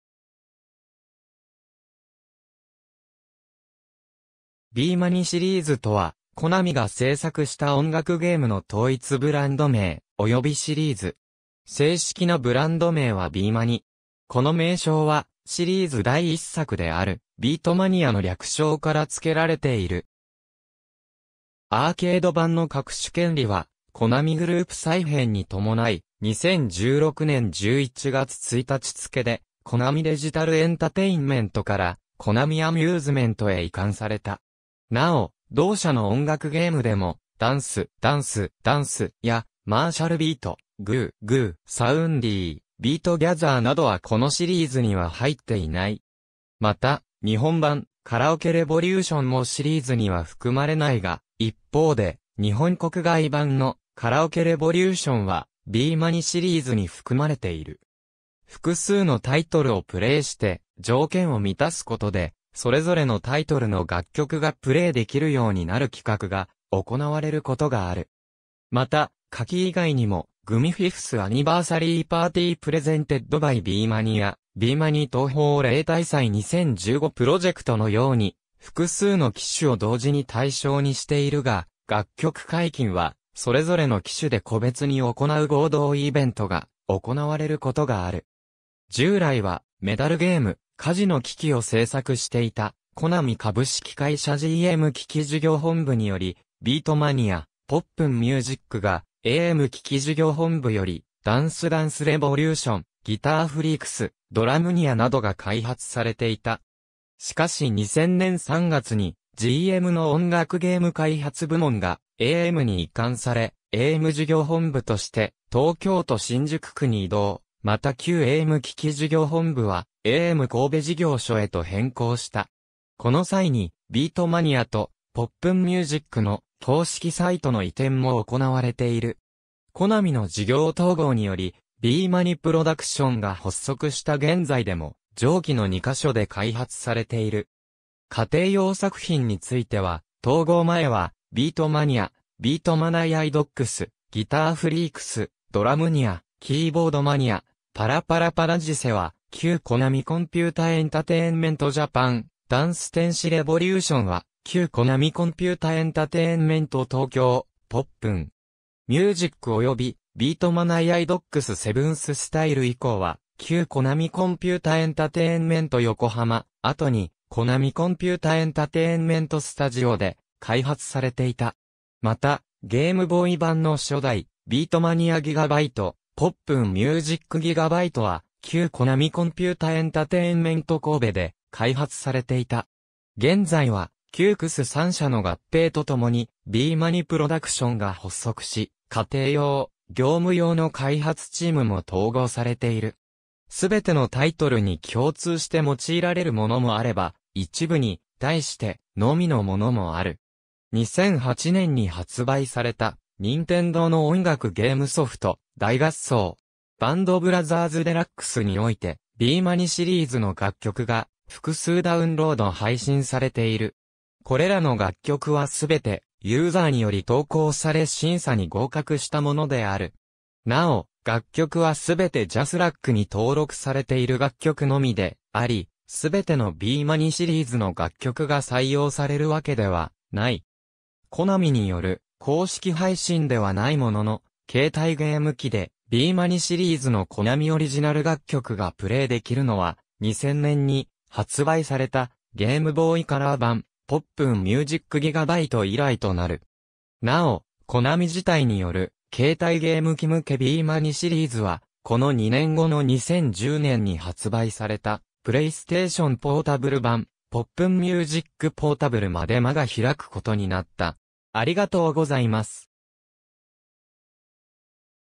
BEMANIシリーズ
BEMANIシリーズ（ビーマニシリーズ）とは、コナミ（コナミアミューズメント・ビーマニプロダクション）が製作した音楽ゲームの統一ブランド名、およびシリーズ。
正式なブランド名はBEMANI（ビーマニ）。
この名称はシリーズ第一作である『beatmania（ビートマニア）』の略称から付けられている。
アーケード版の各種権利は、コナミグループ再編に伴い、2016年11月1日付でコナミデジタルエンタテインメントからコナミアミューズメントへ移管された。
なお、同社の音楽ゲームでも『Dance!Dance!Dance!』や『MARTIAL BEAT』、『Goo! Goo! SOUNDY』、『beat gather』などはこのシリーズには入っていない。
また、日本版『カラオケレボリューション』もシリーズには含まれないが、一方で日本国外版の『Karaoke Revolution』はBEMANIシリーズに含まれている。
複数のタイトルをプレイして条件を満たすことで、それぞれのタイトルの楽曲がプレイできるようになる企画が行われることがある。
また下記以外にも、「GUMI 5th Anniversary party Presented by BEMANI」や「BEMANI×TOHO REITAISAI 2015 project」のように、複数の機種を同時に対象にしているが楽曲解禁はそれぞれの機種で個別に行う合同イベントが行われることがある。
従来はメダルゲーム、カジノ機器を制作していたコナミ株式会社GM機器事業本部（神奈川県座間市）により『beatmania』、『pop'n music』が、AM機器事業本部（兵庫県神戸市西区・西神工業団地）より『Dance Dance Revolution』、『GuitarFreaks』、『drummania』などが開発されていた。
しかし2000年3月に、GMの音楽ゲーム開発部門がAMに移管され、AM事業本部として東京都新宿区に移動（後に港区六本木の六本木ヒルズ→同赤坂の東京ミッドタウンに移動）、また旧AM機器事業本部はAM神戸事業所へと変更した。
コナミの事業統合により「ビーマニプロダクション」が発足した現在でも上記の二ヵ所で開発されている。
家庭用作品については、統合前は『beatmania』、『beatmaniaIIDX』、『GUITARFREAKS』、『drummania』、『KEYBOARDMANIA』、『ParaParaParadise』は旧コナミコンピュータエンタテインメントジャパン、（渋谷区恵比寿の恵比寿ガーデンプレイス→六本木ヒルズ→ミッドタウン）『DanceDanceRevolution』は旧コナミコンピュータエンタテインメント東京（千代田区神田神保町→中央区晴海の晴海アイランドトリトンスクエア→ミッドタウン）、『pop'n
music』及び『beatmaniaIIDX 7thStyle』以降は旧コナミコンピュータエンタテインメント横浜、後にコナミコンピュータエンタテインメントスタジオ（六本木ヒルズ→ミッドタウン）で開発されていた。
また、ゲームボーイ版の初代『beatmania GB』、『pop'n music GB』は旧コナミコンピュータエンタテインメント神戸で開発されていた。
現在は、旧KCE3社の合併とともに、「ビーマニプロダクション」が発足し、家庭用・業務用の開発チームも統合されている。
全てのタイトルに共通して用いられるものもあれば、一部に対してのみのものもある。
2008年に発売された任天堂の音楽ゲームソフト『大合奏!バンドブラザーズDX』（ニンテンドーDS）において、BEMANIシリーズ（『beatmania』シリーズ、『pop'n music』シリーズが中心）の楽曲が複数ダウンロード配信されている。
これらの楽曲は全て、ユーザーにより投稿され審査に合格したものである。
なお、楽曲はすべてJASRACに登録されている楽曲のみであり、全てのBEMANIシリーズの楽曲が採用されるわけではない。
コナミによる公式配信ではないものの、携帯ゲーム機でBEMANIシリーズのコナミオリジナル楽曲がプレイ出来るのは、2000年に発売されたゲームボーイカラー版『pop'n music GB』以来となる。
なお、コナミ自体による携...